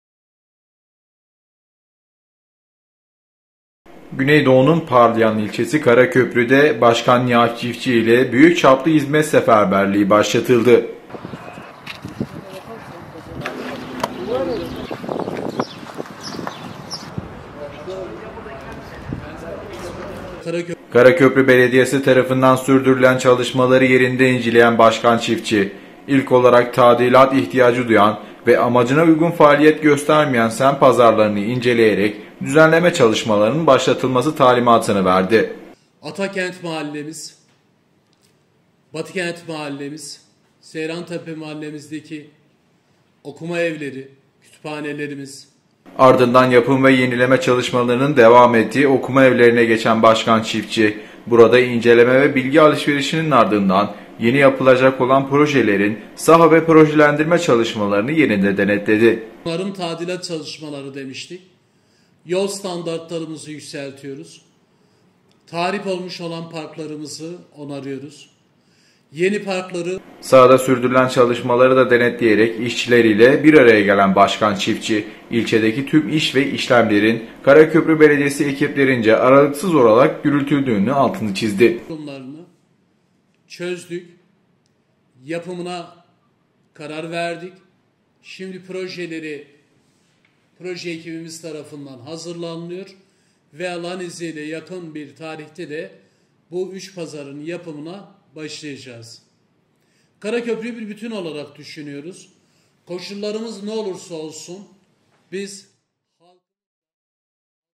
Şanlıurfa’nın O İlçesinin Çehresi Değişiyor!
Karaköprü’nün çehresini değiştirecek olan çalışmaların öncülüğünü yapan Karaköprü Belediye Başkanı Av. Nihat Çiftçi, “Karaköprü Belediyesi olarak tüm ekiplerimiz ile birlikte sahadayız. Semt pazarlarından yeni yollara, 1500 Kişilik Kent Kütüphanesinden Gastronomi Merkezine kadar birçok çalışmayı başlatıyoruz” dedi